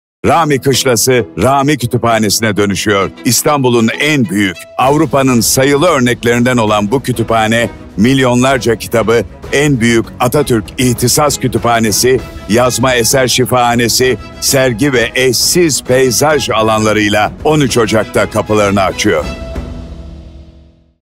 Kültür ve Turizm Bakanlığı (Türkiye Turizm Tanıtım ve Geliştirme Ajansı Genel Müdürlüğü)/Kamu Spotu
13 Ocak 2023 tarihinde açılacak olan İstanbul Rami Kütüphanesinin tanıtımı ve kamuoyunun konuyla ilgili bilgilendirilmesi hakkında kamu spotu.